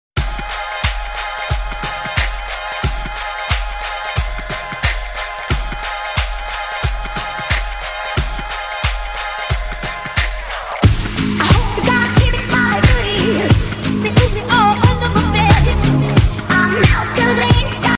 Streams?
Should I start streaming?  As it said in the animation, I would mostly be able to stream on Wednesday... so idk, it could be fun or not.  Who knows!  So just lemme know in the comments.